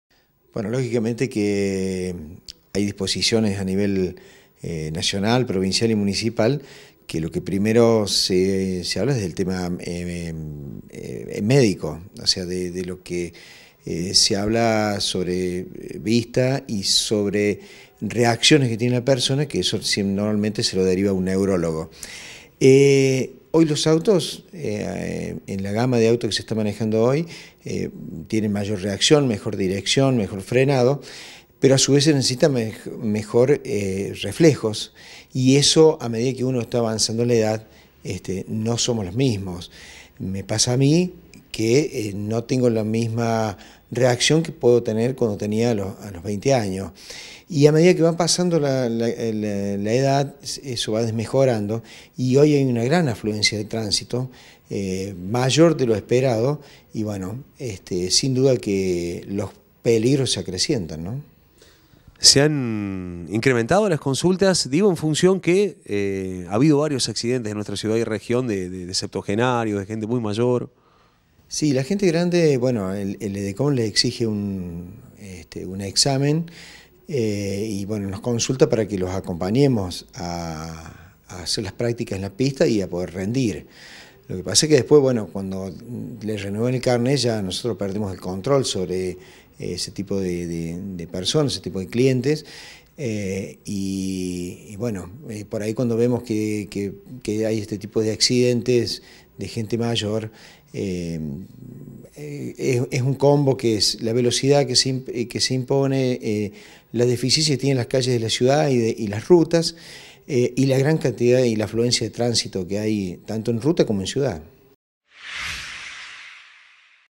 Seguridad vial: recomendaciones para adultos mayores
Luego del accidente del miércoles, en el que falleció un septuagenario conductor, dialogamos con Norberto Ferrero, titular de una escuela de conducción segura.